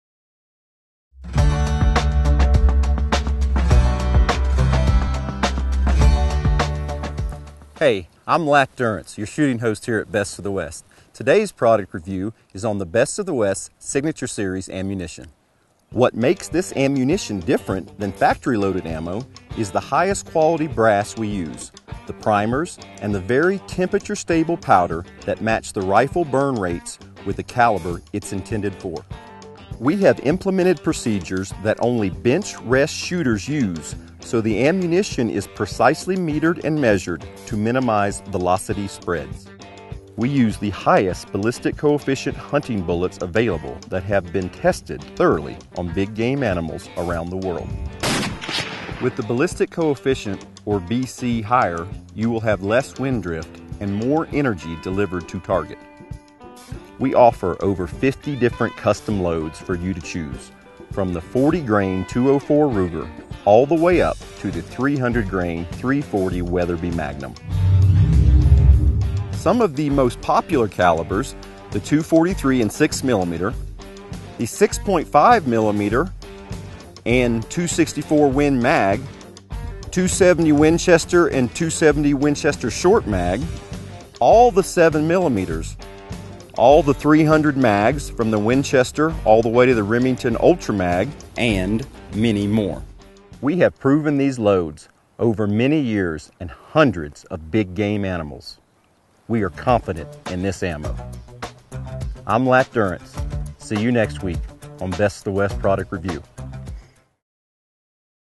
Product Review- Signature Series Ammunition by The Best of the West
The best brass in the industry. Extremely high BC bullets. Temperature stable powder. Solid primers. The finest hunting ammunition out there!